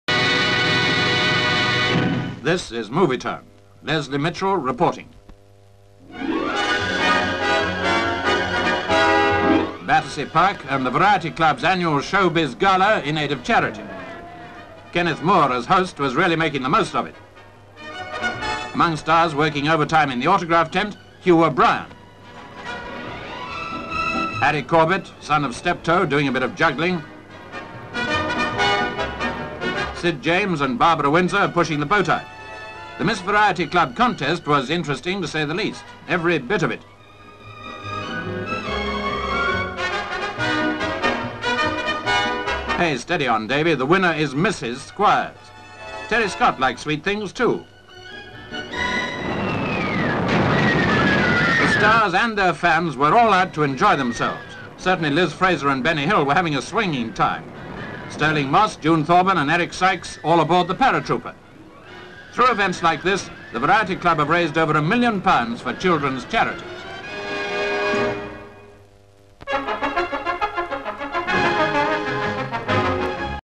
STARS ON PARADE
(16 May 1963) Battersea Park, and the Variety Club's annual 'showbiz-gala' in aid of charity. Kenneth More, as host was really making the most of it. Some of the stars taking part were, Hugh O'Brien in the autograph tent, Harry Corbett, son of Steptoe, Sid James, and Barbara Windsor. The 'Miss Variety Club' contest was interesting and the winner was Mrs Squires. Through events like this, the Variety Club have raised over a million pounds for children's charities.